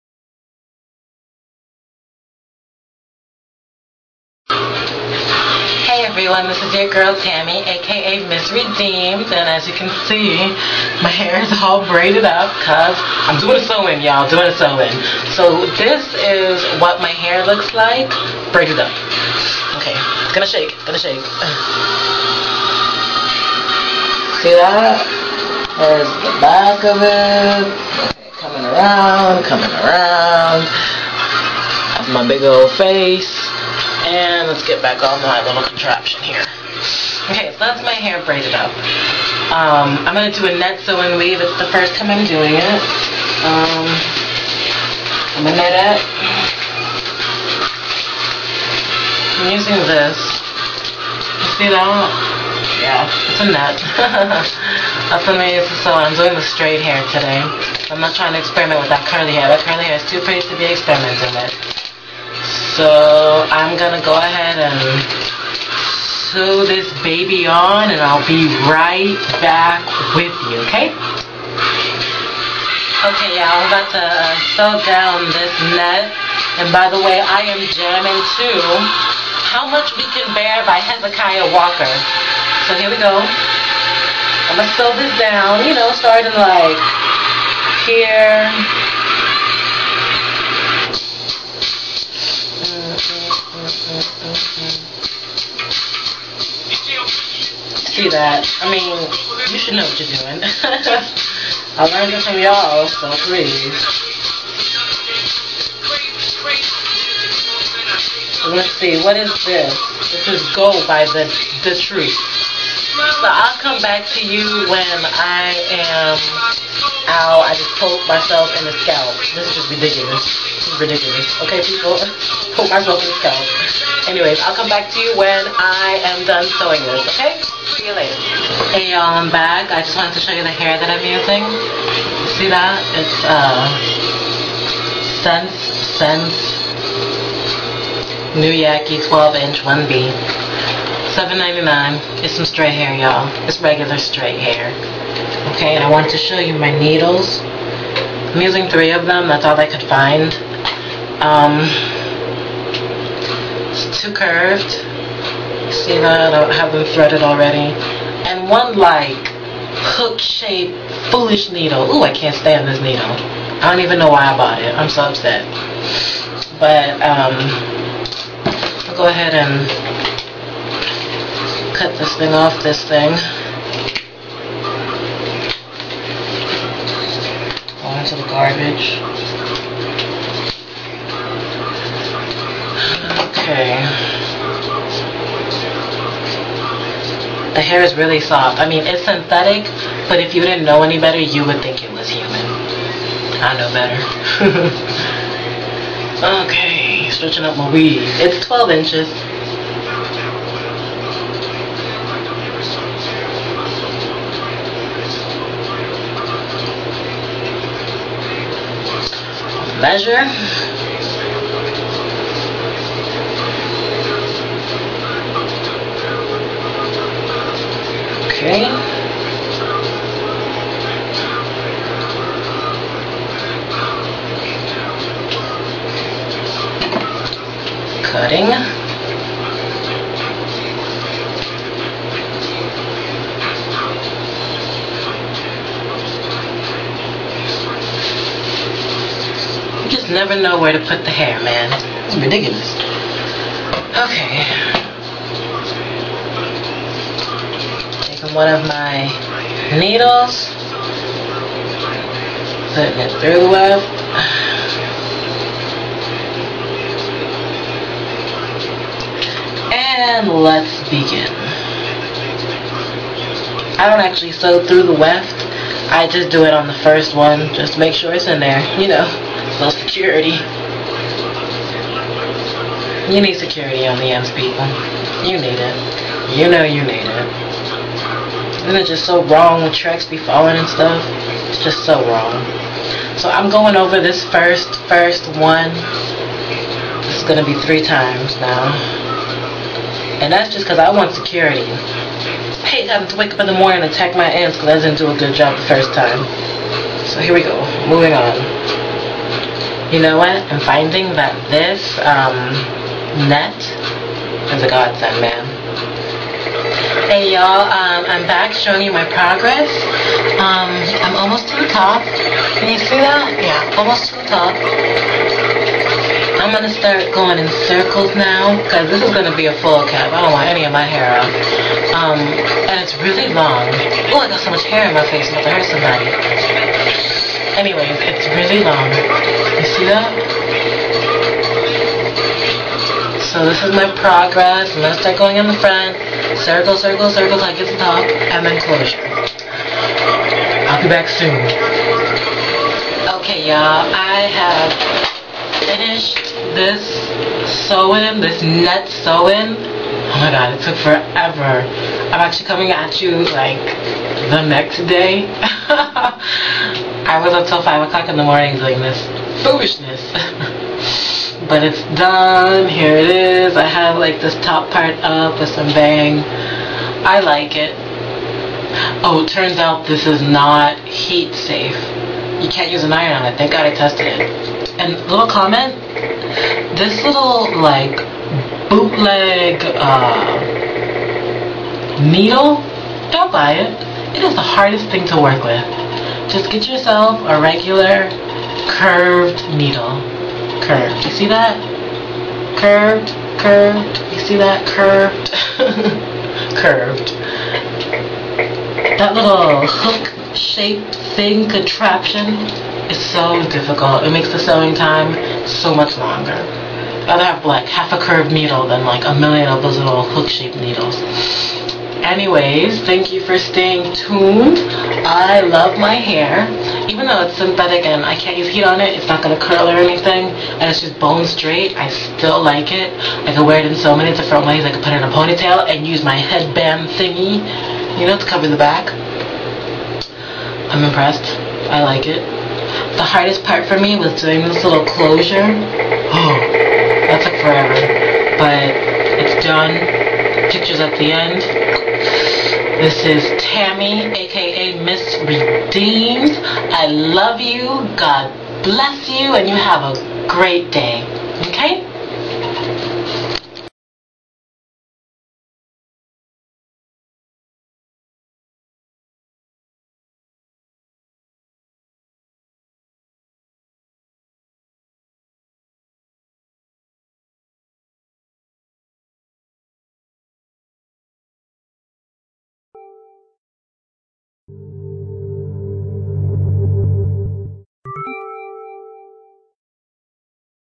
My First Net Sew In Weave
This is a net sew in weave.  The hair is synthetic.  12 inch. 1b.  It took forever ya'll.  Enjoy.